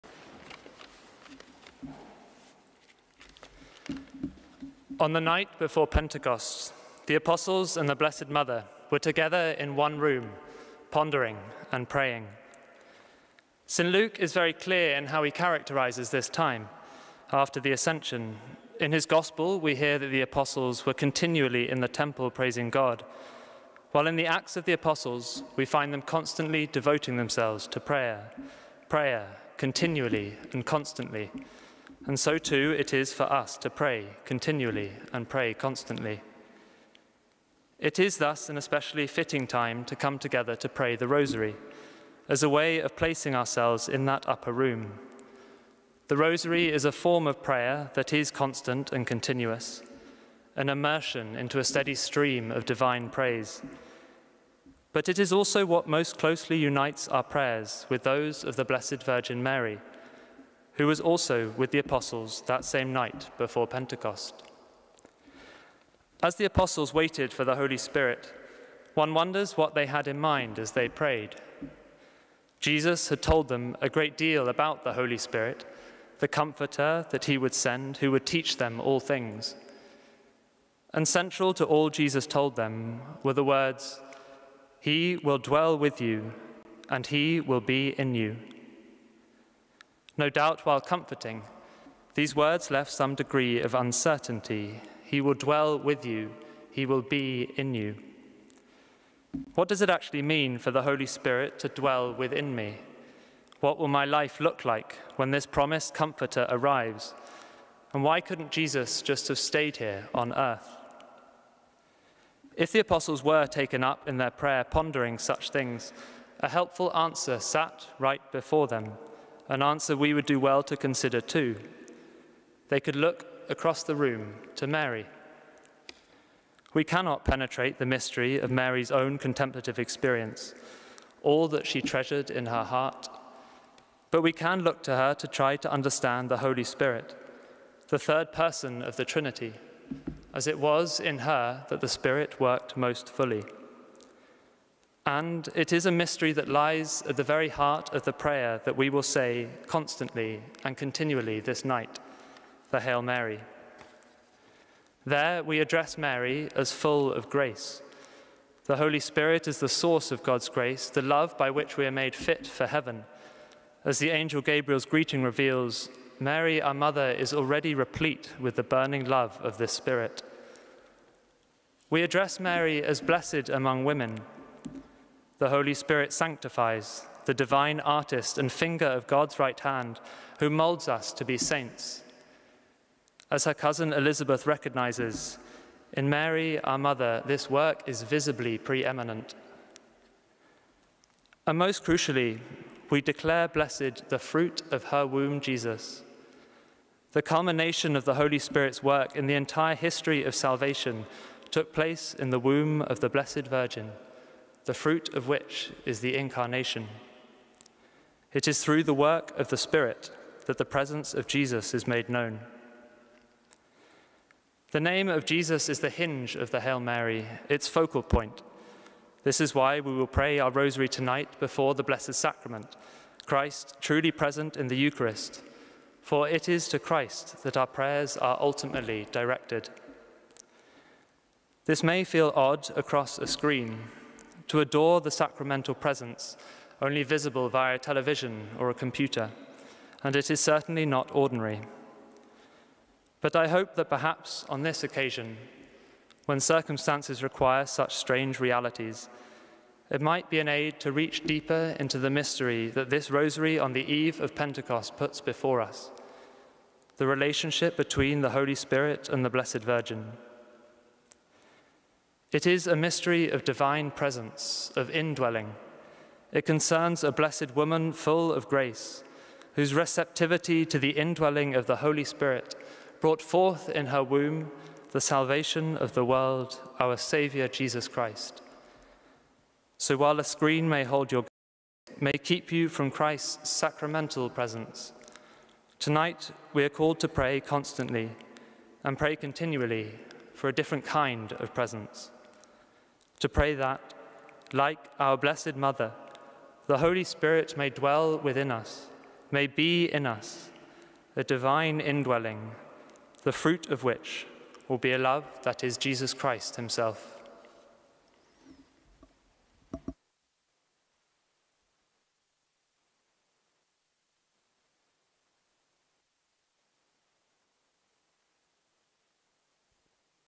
Blessed Is the Fruit of Her Womb - Br John Church OP
A reflection given at the Blackfriars Rosary Vigil on the eve of Pentecost (30.05.20), by Br John Church OP.